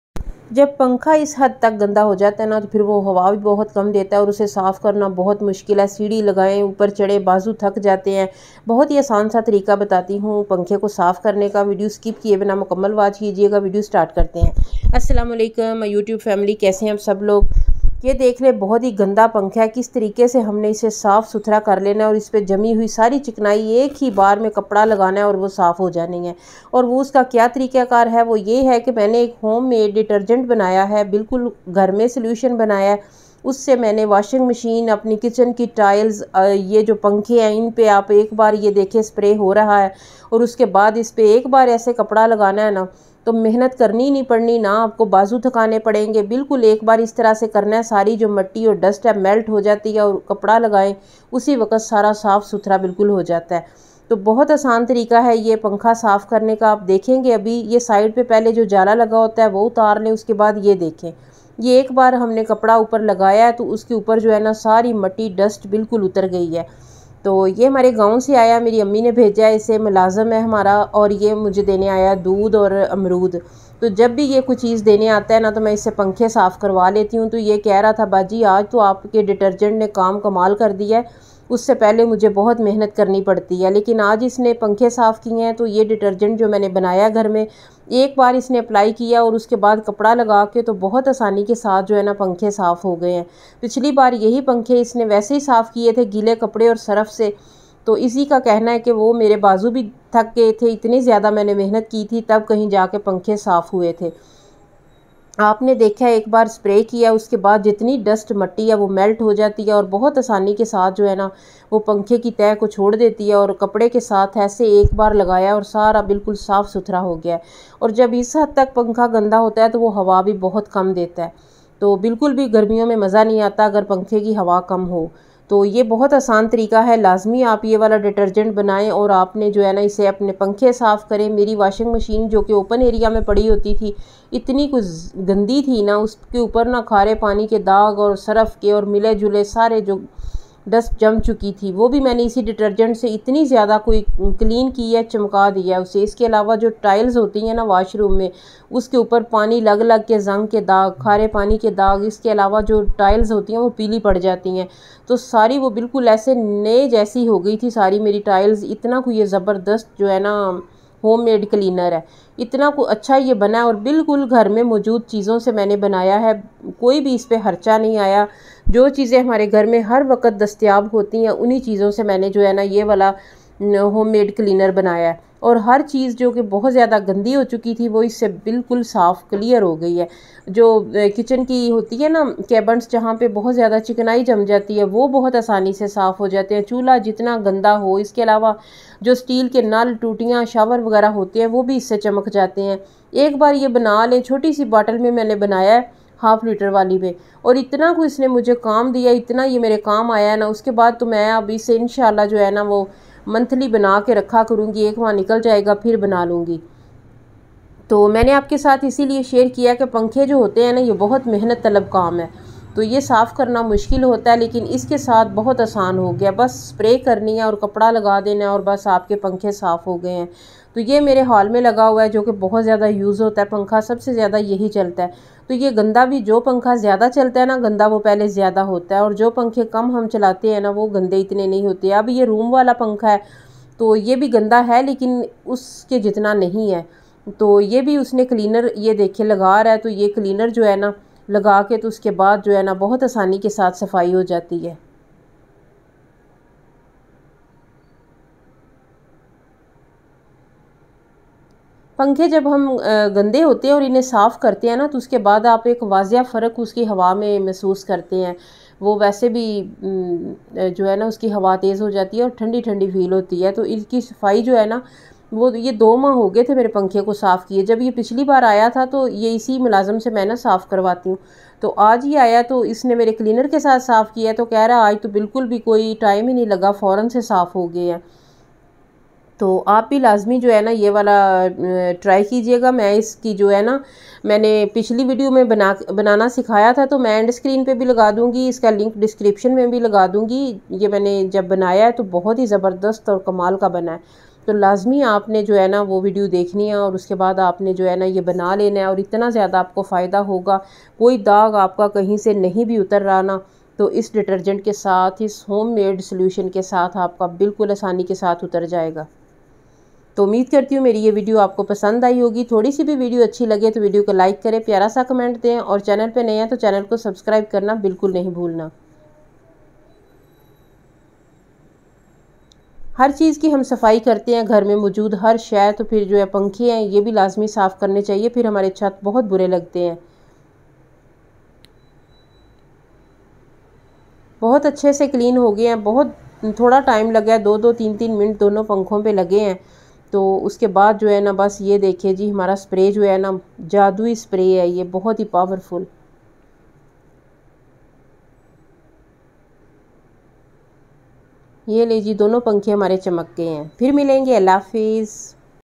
Easiest way to Clean Ceiling Fans | How to clean a metal fan | Cleaning Tips | kitchen tips
There is a very simple way to clean the fan. I have made a homemade cleaner that easily cleans washroom tiles and washing machine stains, as well as kitchen grease. Fanke Hai Jimmy Dust cleans in one go